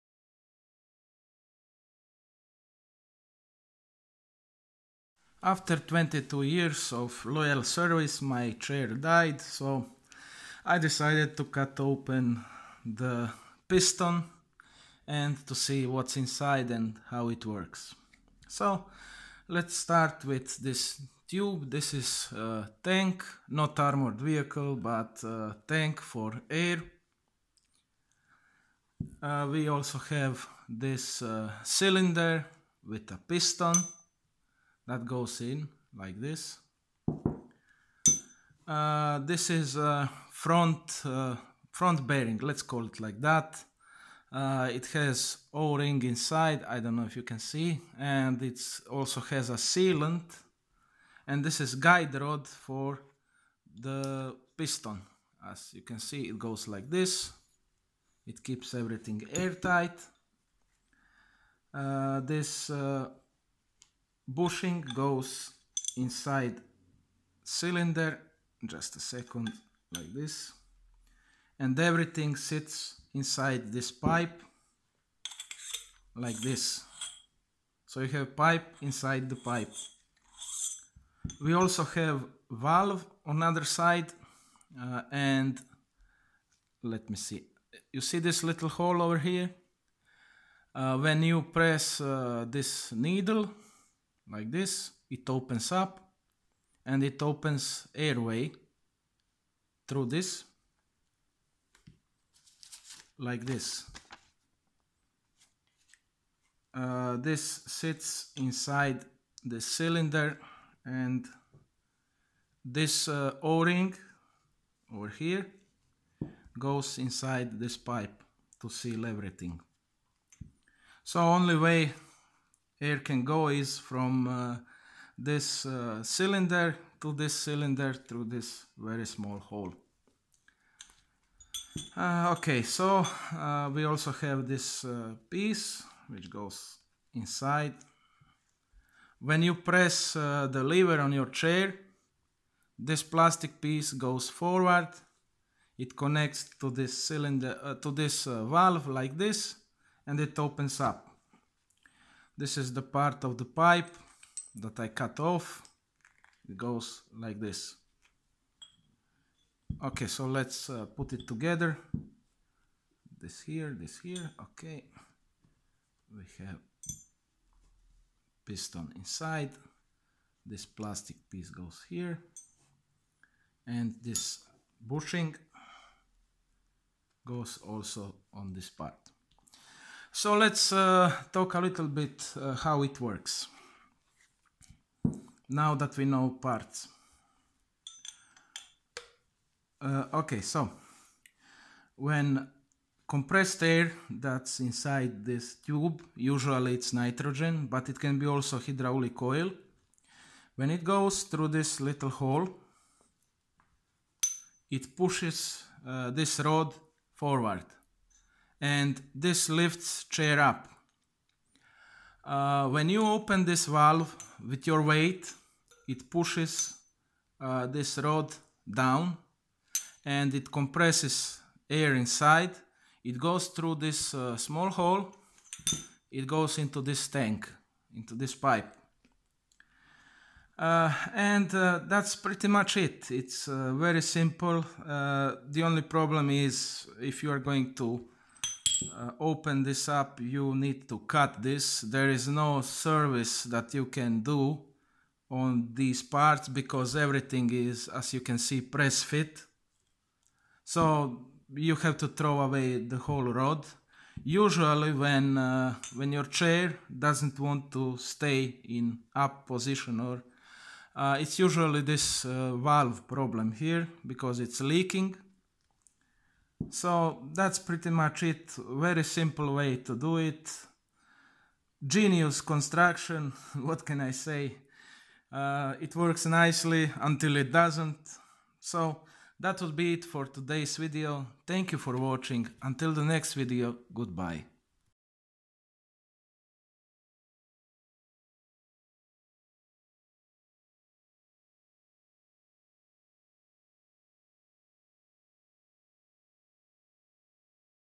How pneumatic chair lifter works
Ever wondered what kind of magic lifts your chair up ?
Watch my video and find out !